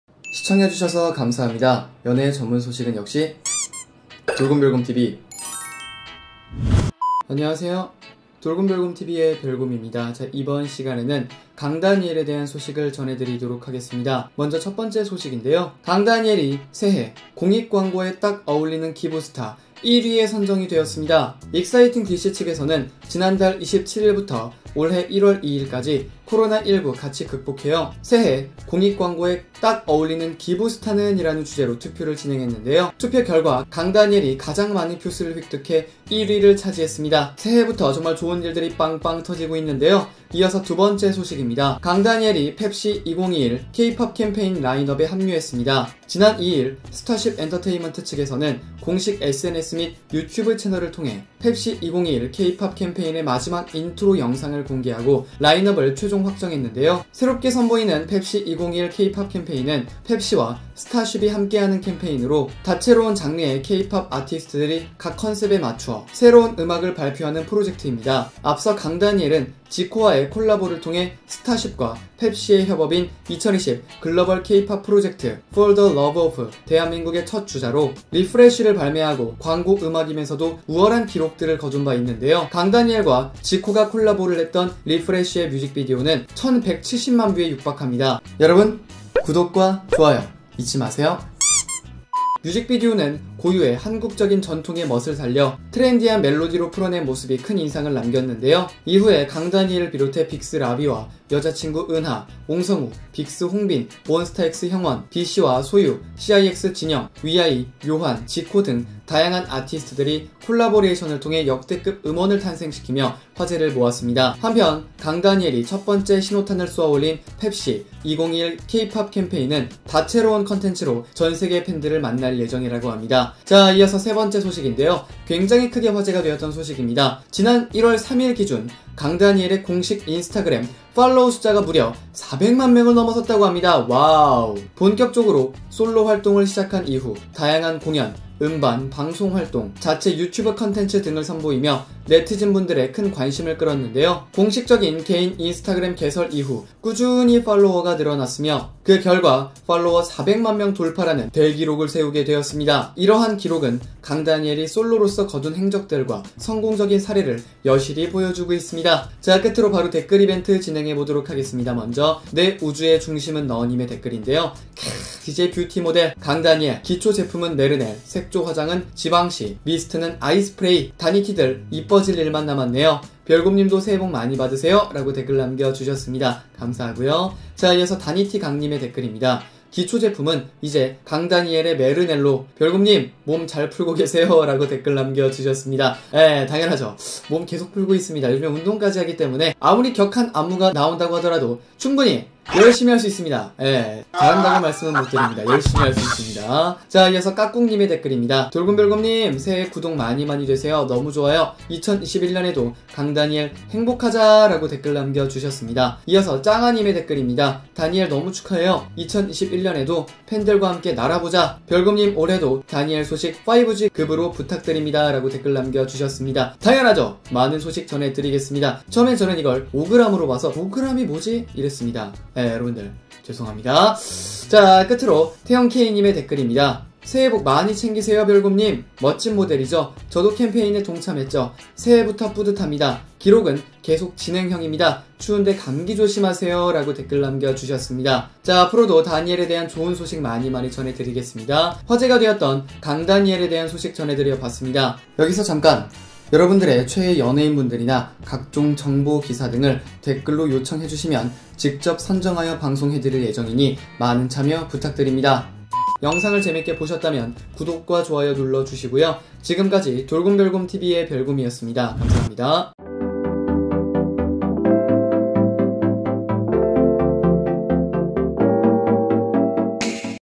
【ENG】실화? 강다니엘, 400만 팔로워 돌파 대박!! +새 음악프로젝트 예고? Kang Daniel 돌곰별곰TV
정대규

감사합니다.

네이버 파파고 다국어 번역 :

Really? Kang Daniel, we have more than 4 million followers! Awesome! +New music project preview?

実話ですか？カン·ダニエル、 400万フォロワー突破！すごい！ +新しい音楽プロジェクトの予告ですか？

真的嗎？姜丹尼爾，突破400萬粉絲 大發！ +新音樂項目預告？

감사합니다^____^

감사합니다.